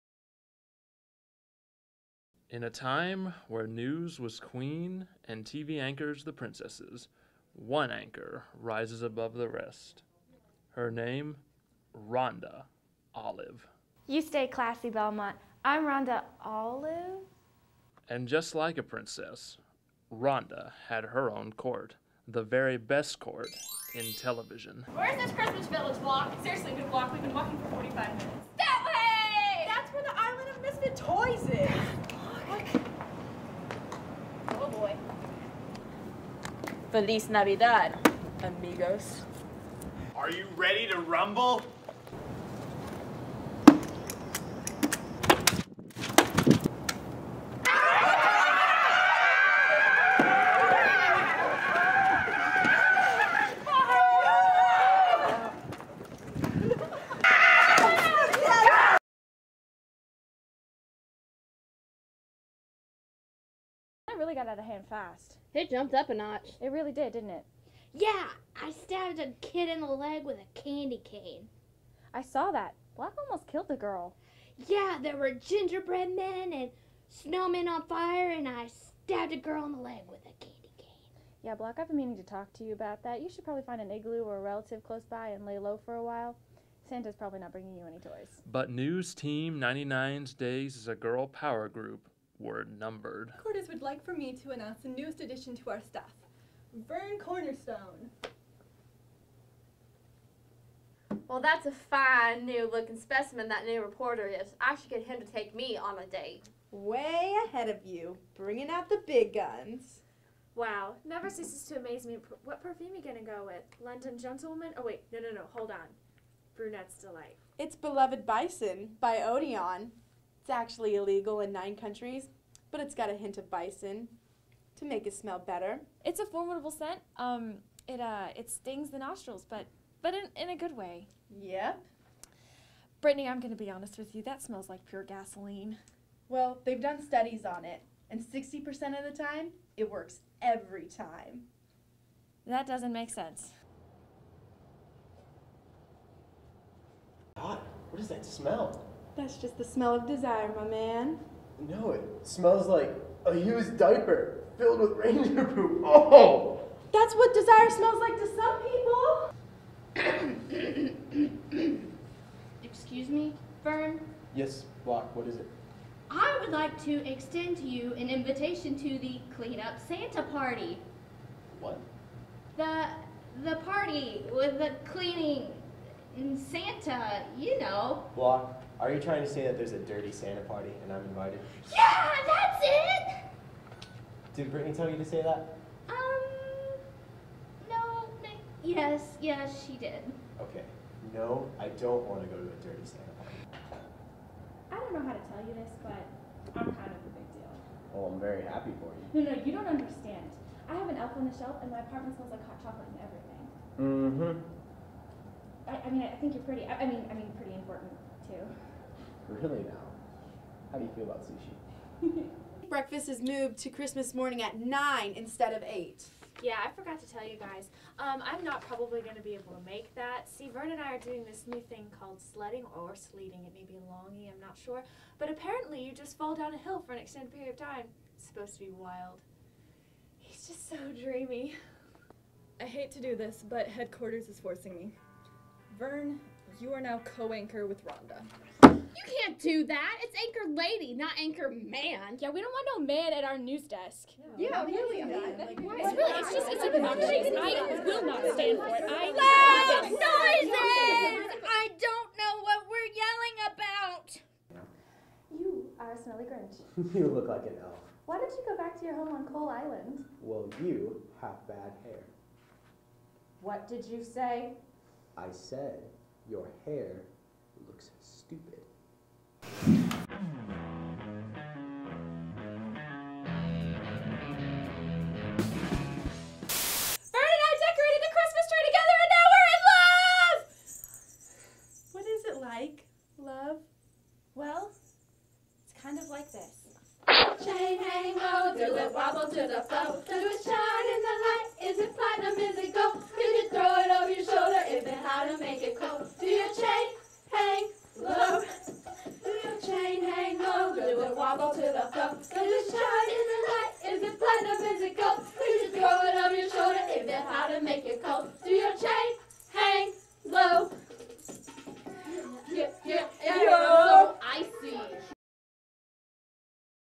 Anchorladies: The true story of Rhonda Olive
It's our Christmas gift to you:

There was a time when newsrooms were a woman's domain. And the top anchor during that period was Rhonda Olive and her News Team 99. But all that changes with the arrival of new reporter of Vernon Cornerstone.